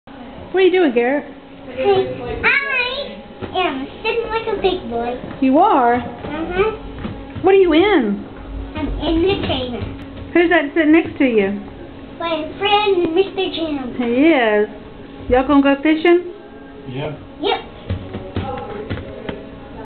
Garrett Witwer and Mr. Jim in the Chamber
Garrett's friend Mr. Jim talk about the Chamber